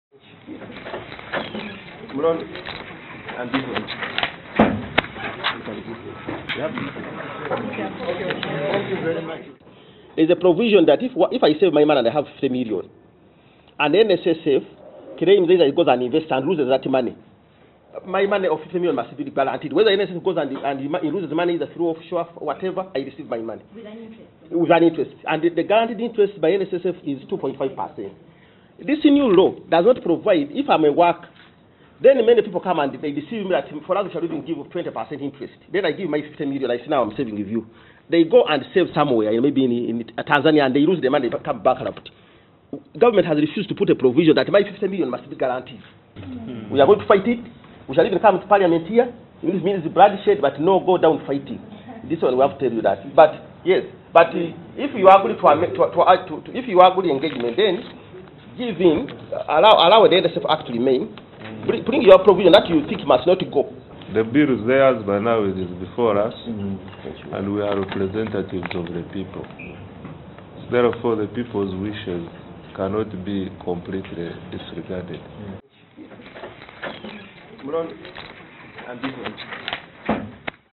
Trade unions reject proposals to amend Pension and National Social Security Fund Act
Workers under the Central Organization of Free Trade Union have rejected government’s proposals to amend the Pension and National Social Security Fund Act saying if it is liberalised, many workers risk losing their money to private entities if they make losses. COFTU’s General Secretary Sam Lyomoki told Parliament’s Finance Committee that many private players might decide to declare their entities bankrupt and this will affect the savers. COFTU also demands that the current NSSF act be amended so that members can access some of their money once they have worked for a given period of time.